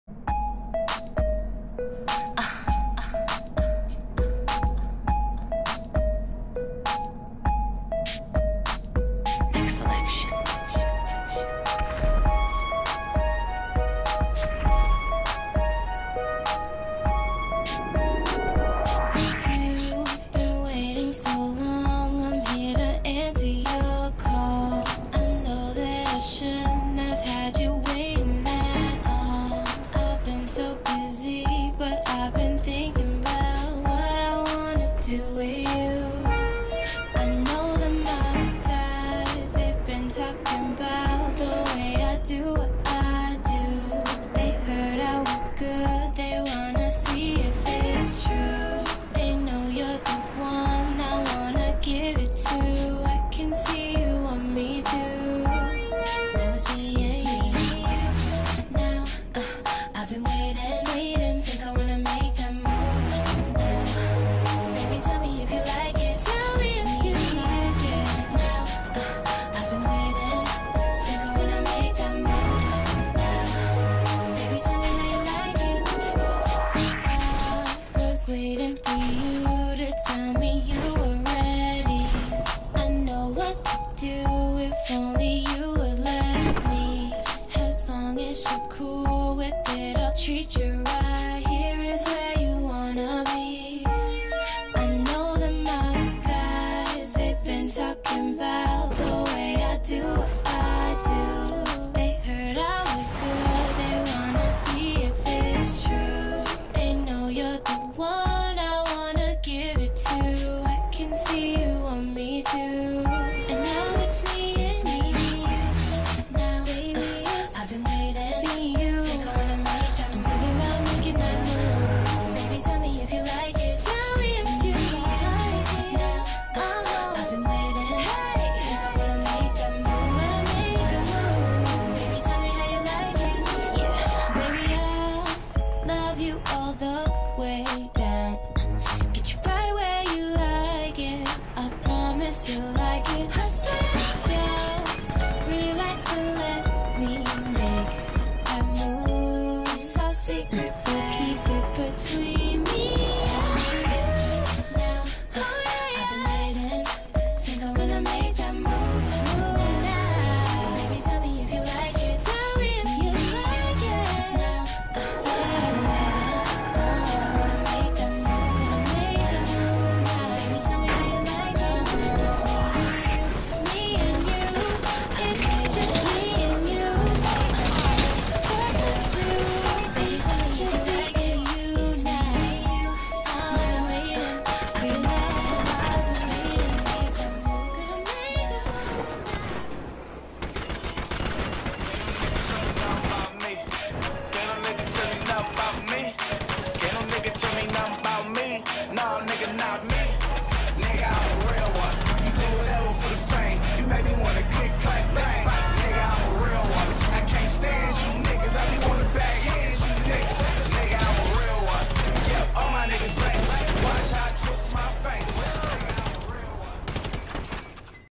Draft Day (PRACTICE)
Just practicing with the editor and learning the camera features. Still so much to learn, I just started working with the editor about two weeks ago. So, this is just a practice video.